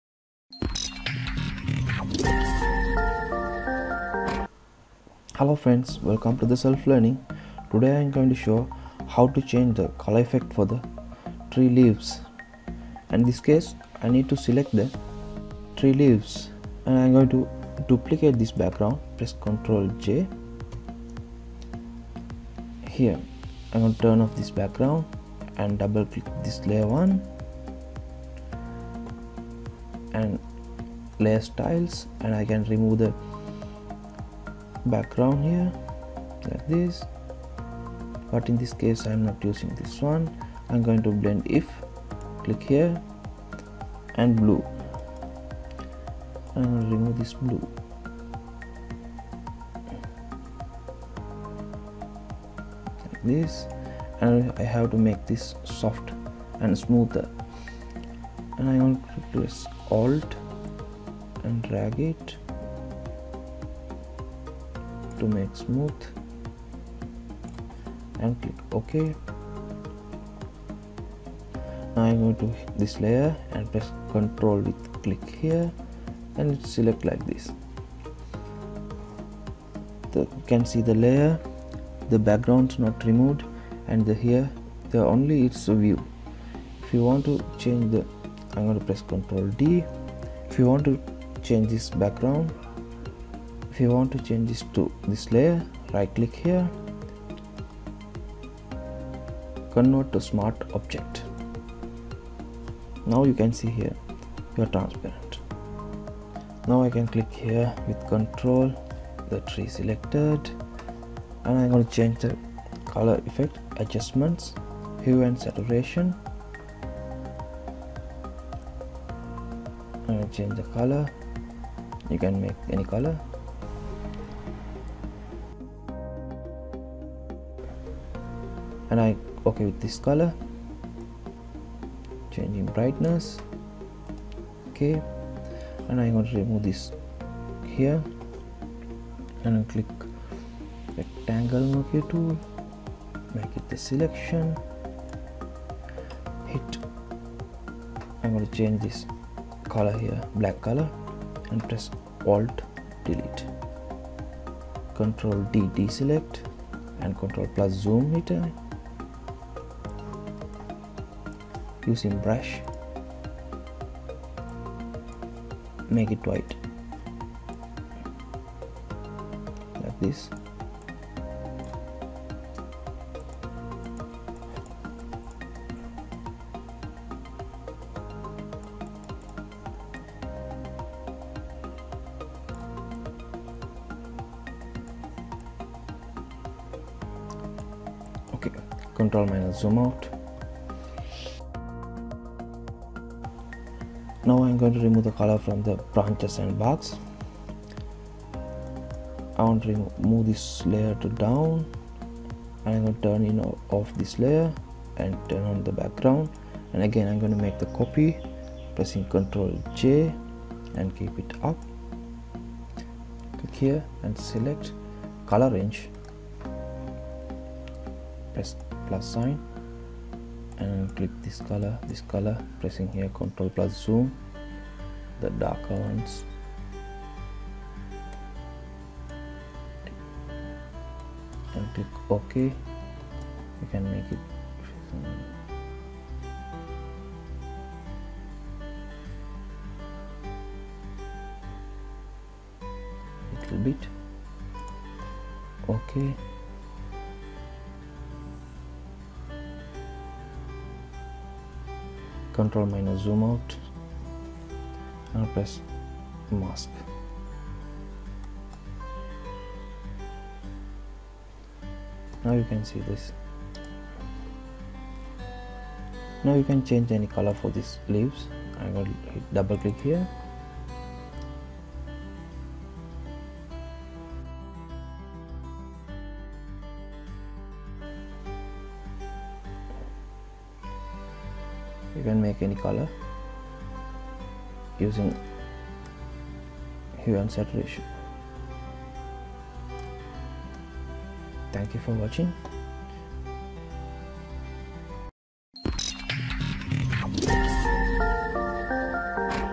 How to change Tree Leaves Color in Photoshop
This Photoshop Tutorial covers How to Select and change Tree Leaves Color in Photoshop  using Selection , masking layer ,Blend if , Hue/ Saturation

Category
Education